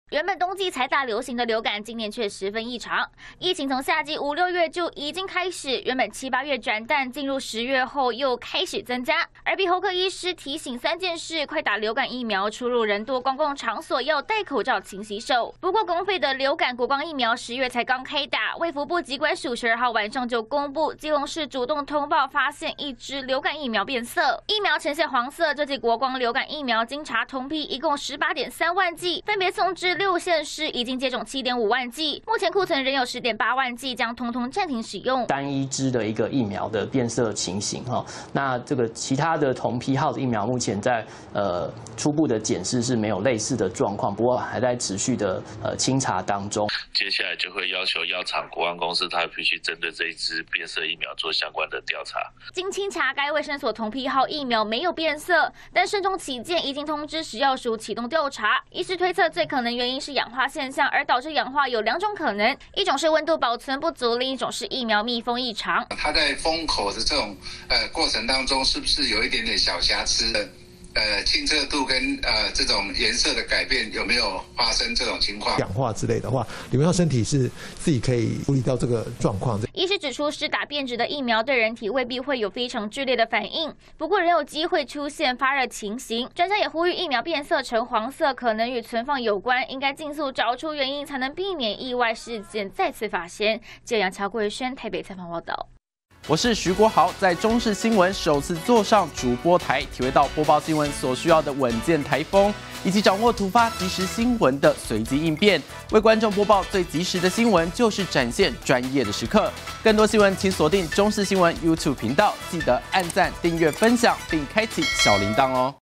流感疫苗出包! 國光疫苗變色 6縣市10.8萬劑停打│中視新聞 20241013
又爆流感疫苗變色異常!疾管署12日晚間緊急宣布，基隆市衛生局通報，1劑國光流感疫苗，外觀出現異常、呈現淡黃色，同批號庫存10.8萬劑暫停使用，但已有7.5萬劑被接種!疾管署表示尚未接獲其他類似通報，也未接獲該批號疫苗接種後不良事件通報，目前是單一個案，週一請國光公司針對疫苗變色進行調查，專家推測應該是保存不當發生氧化。